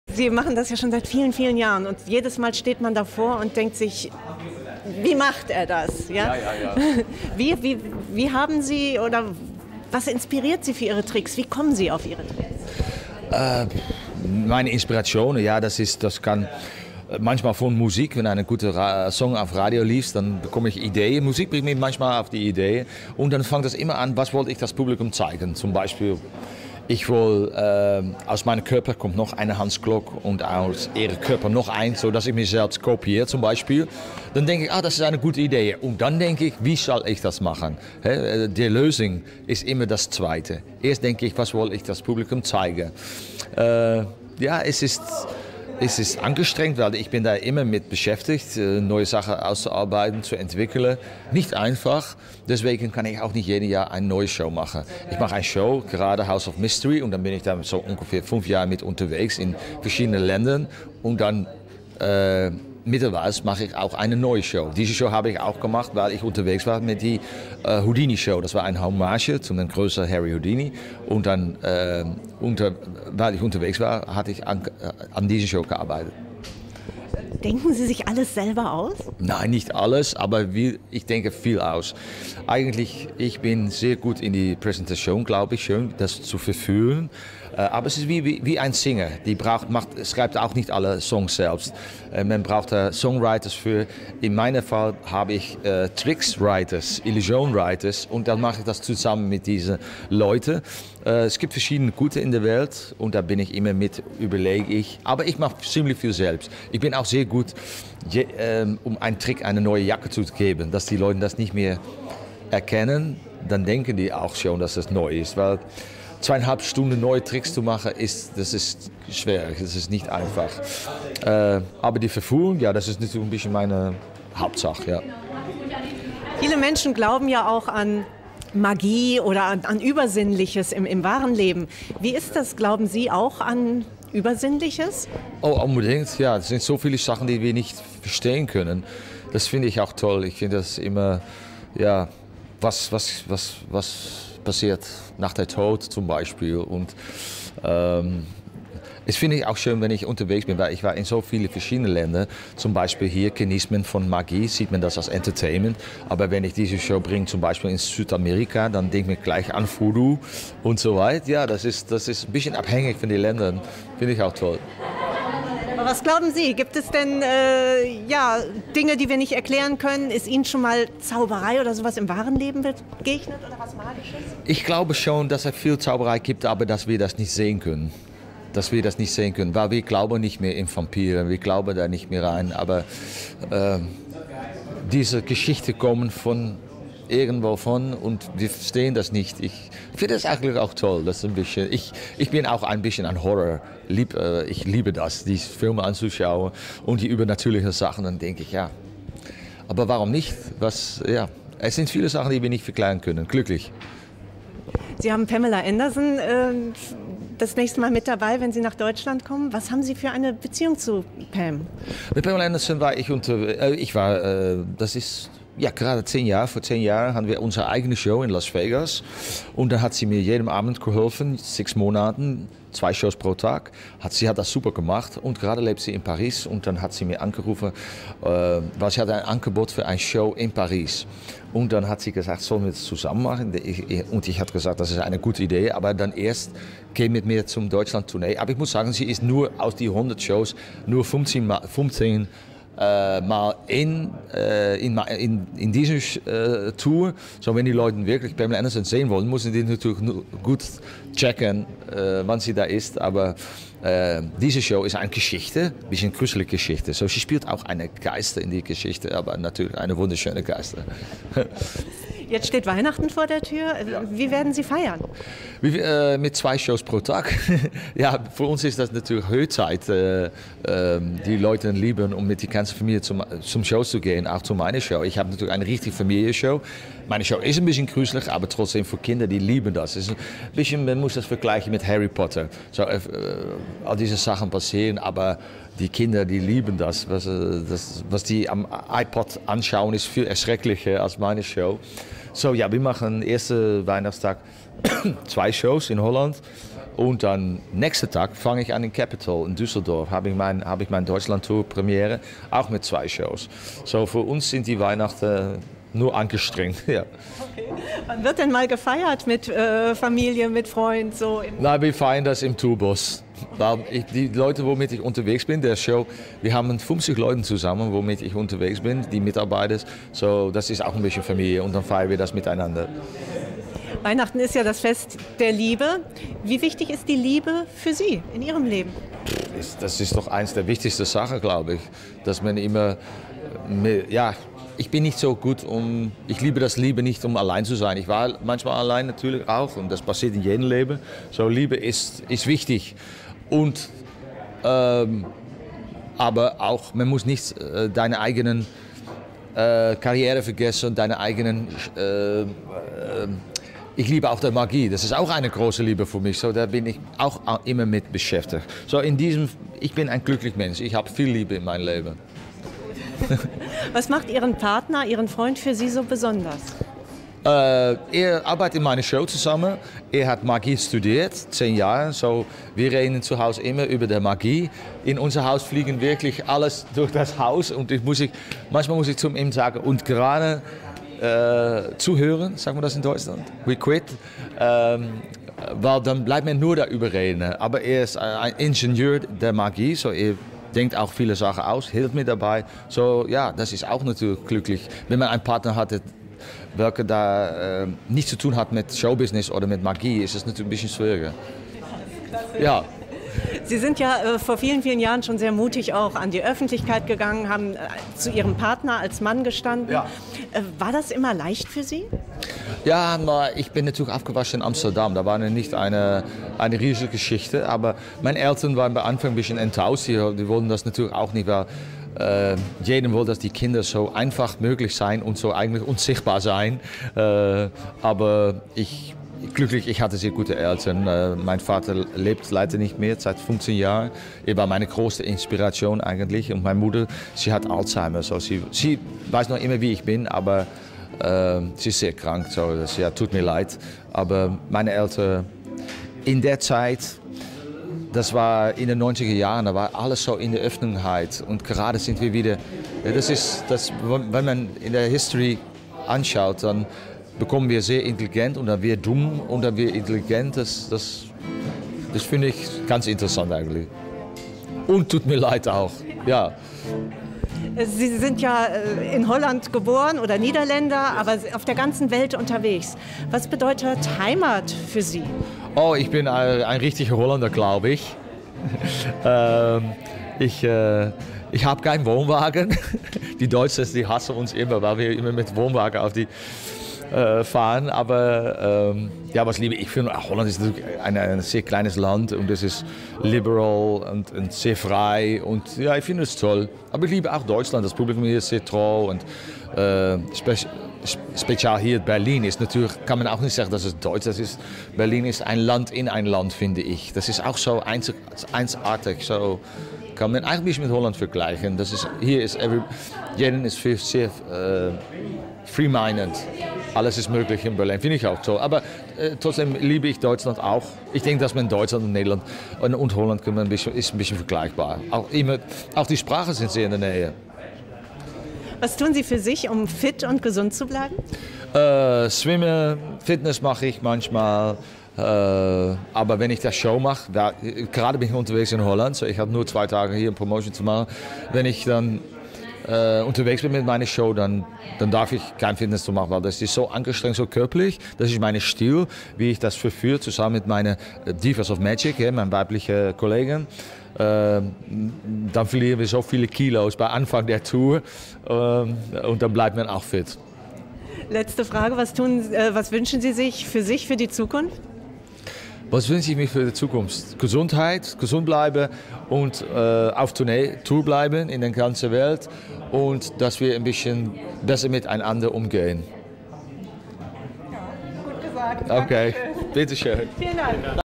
Hans Klok über Magie, seinen neuen Freund und Weihnachten!
Der schnellste Magier im ganz persönlichen Interview. Mehr